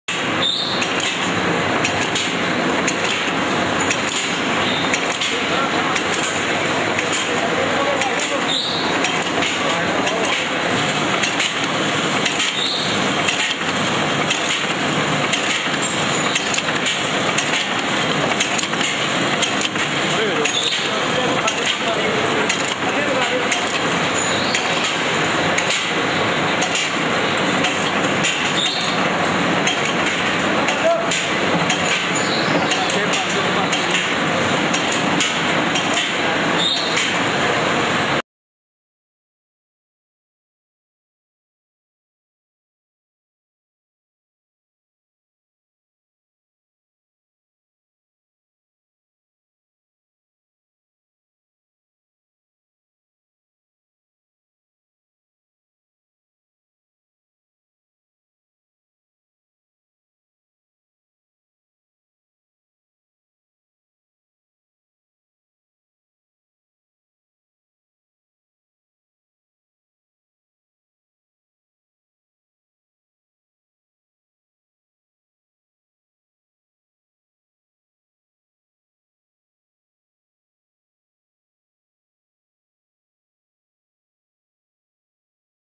Manek - Roofing Nail Making Machine Model: RNM-100 making Twisted Stem Nails
Umbrella  / Roofing Nail Making Machine
Maquinaria para fabricar Clavos de Techar / Clavos Paraguas / Clavos para Calaminas

Maneklal Global Exports
67 Bajaj Bhavan, Nariman Point, Mumbai - 400021, India